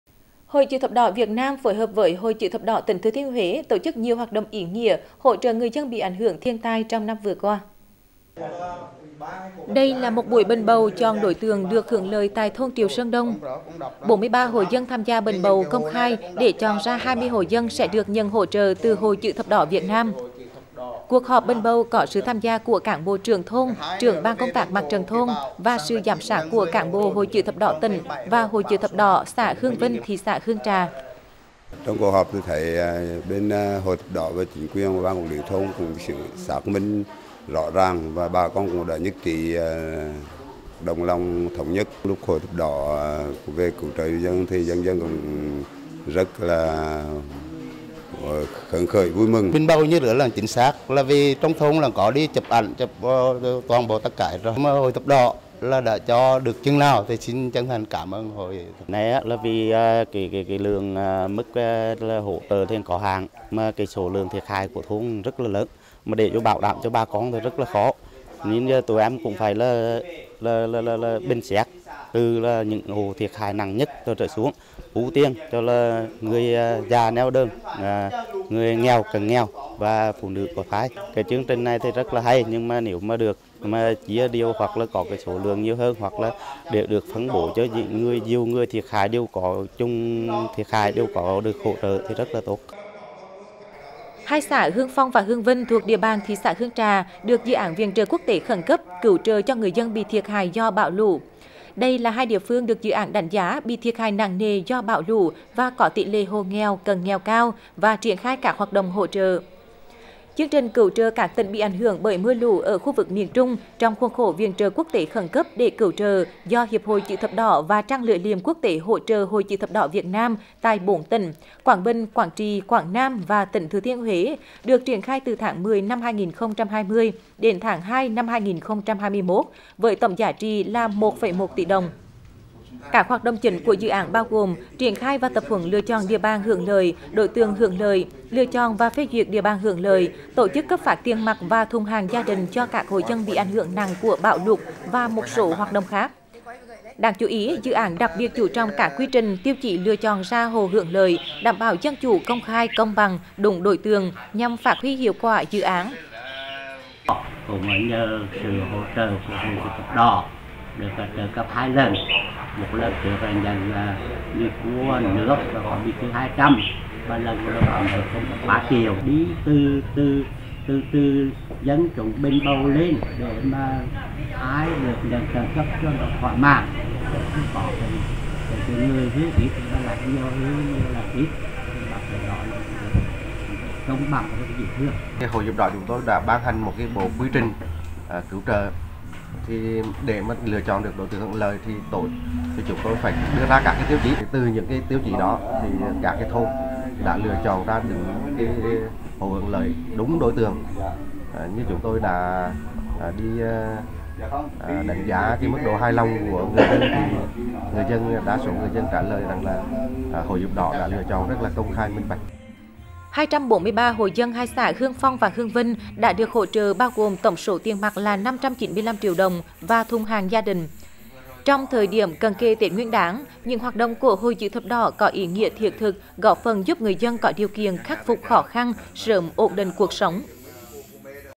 Gần 300 triệu hỗ trợ 243 hộ bị ảnh hưởng thiên tai
Hội chữ thập đỏ Việt Nam phối hợp với Hội chữ thập đỏ tỉnh TT Huế tổ chức nhiều hoạt động ý nghĩa hỗ trợ người dân bị ảnh hưởng thiên tai trong năm vừa qua.